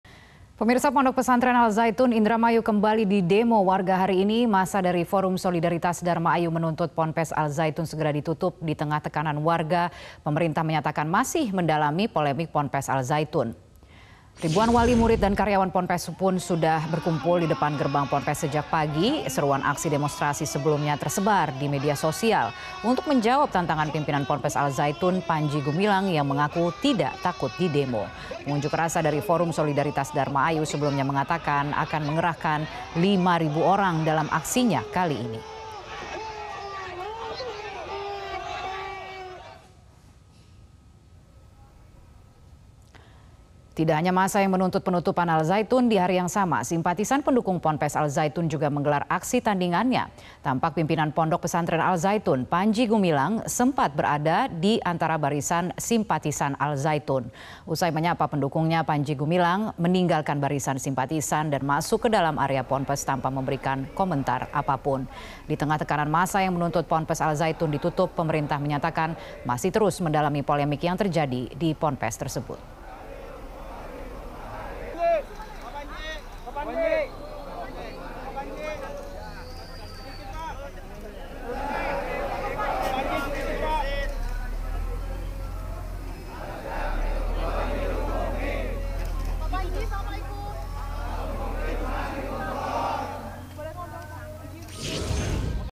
Pimpinan Ponpes Al Zaytun Datangi Simpatisan
MetroTV, Petinggi Pondok Pesantren Al-Zaytun, Panji Gumilang menyambangi para simpatisan Ponpes Al-Zaytun pada Kamis (22/6) pagi. Panji terlihat menyapa para simpatisan Ponpes Al-Zaytun yang sudah berkerumun ditengah adanya informasi mengenai demo yang akan digelar untuk menuntut ditutupnya Ponpes yang ia pimpin tersebut.